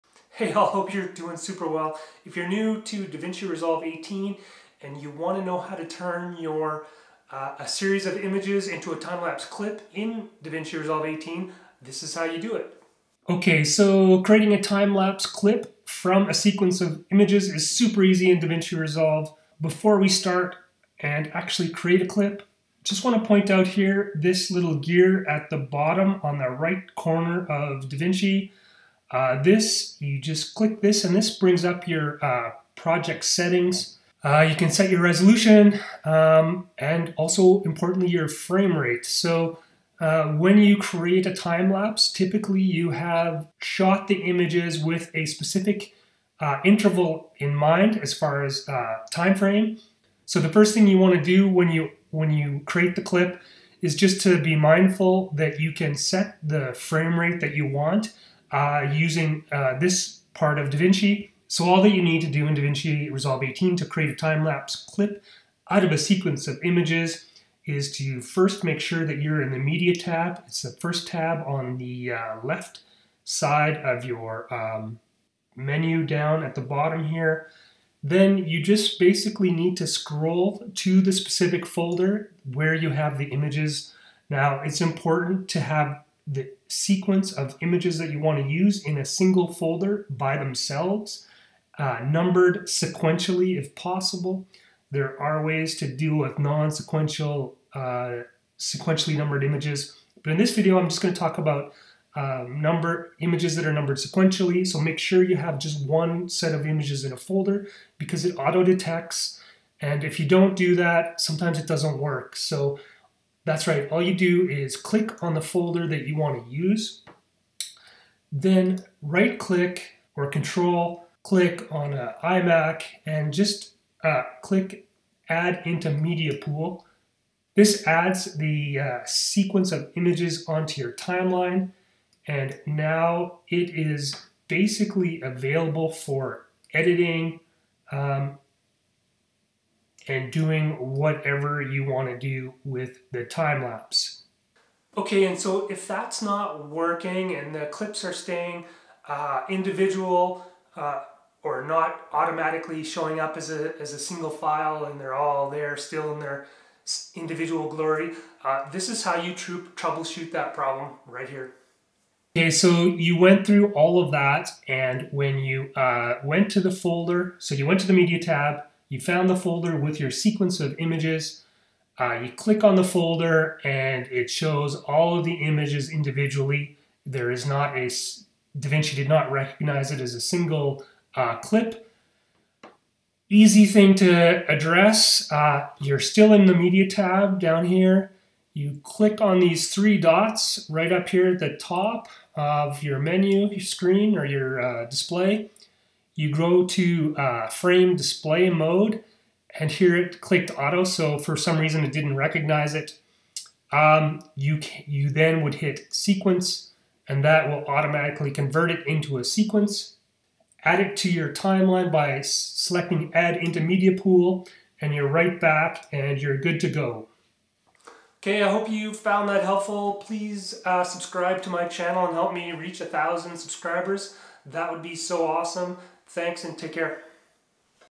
Creating Timelapse Clips in Davinci Resolve 18
Quick video to get you going with creating timelapses clips in DaVinci Resolve 18  and trouble shooting when a sequence is not auto detected by DaVinci Resolve 18.